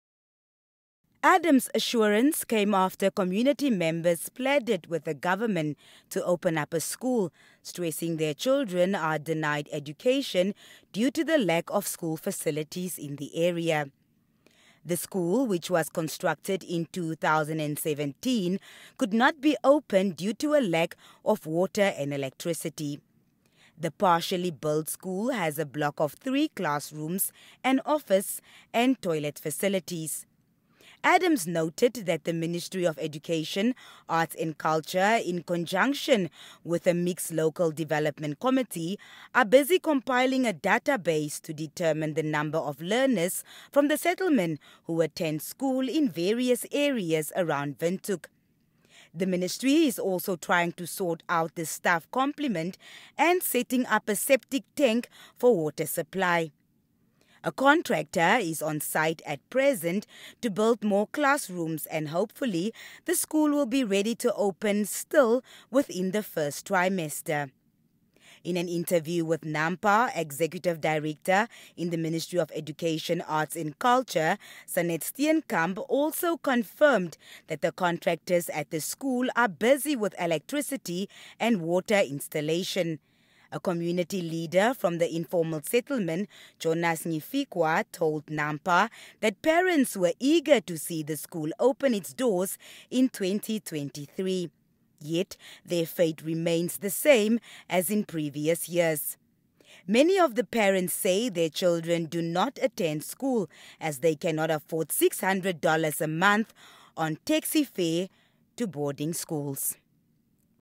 Mix informal settlement residents assured of imminent opening of a primary school- nbc
Windhoek Rural Councilor, Piet Adams, has assured Mix informal settlement residents that a primary school will be opened in the area over the next three months.

Adams' assurance came after community members pleaded with the government to open up a school, stressing that their children are denied an education due to the lack of school facilities in the area.

The school, which was constructed in 2017, could not be opened due to a lack of water and electricity.